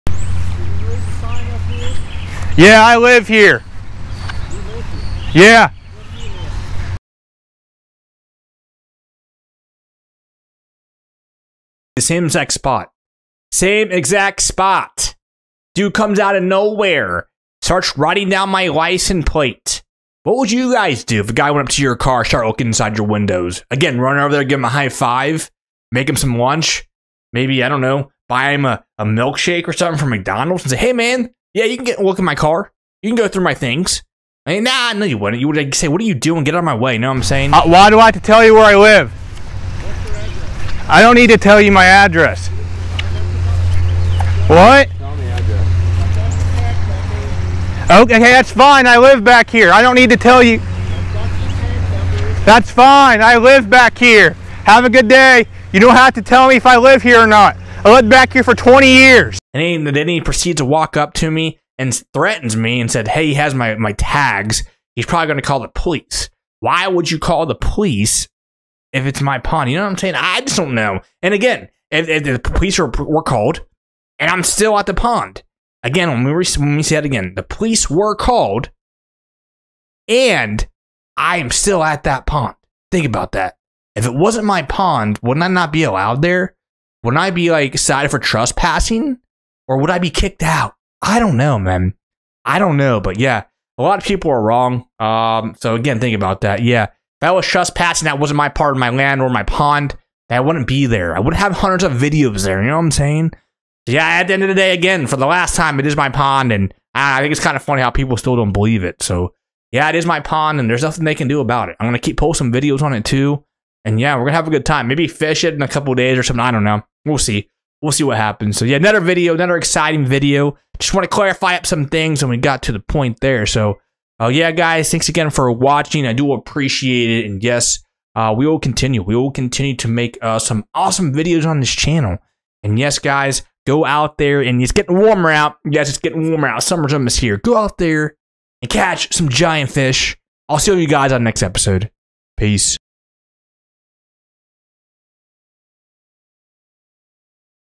Angry Man Tries To Kick Me Out Of My Own Fishing Pond!!! Dude It's My Pond!
He has no idea who I am this is insane!!!